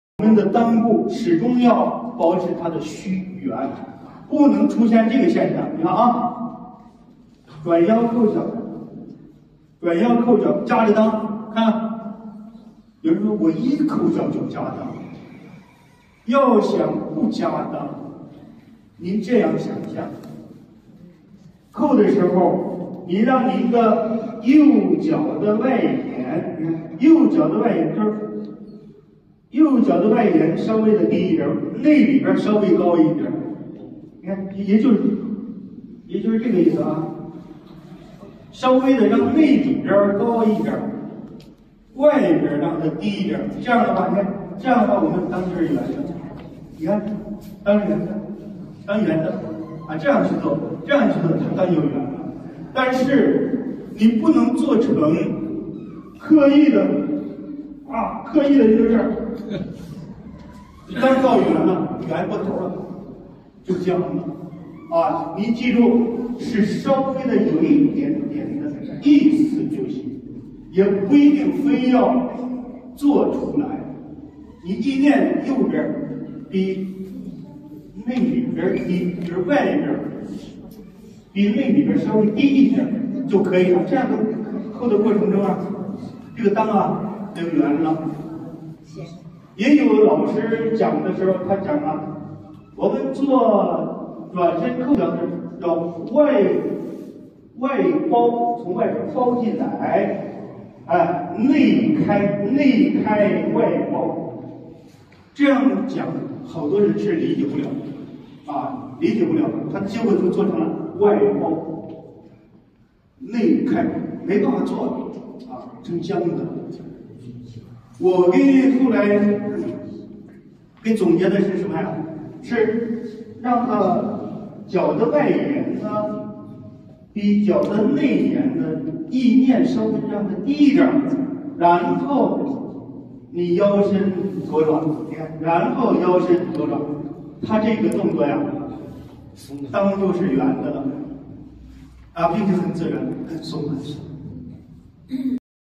圓襠要領。
2024年3月中旬李占英師父在山東授課內容分享！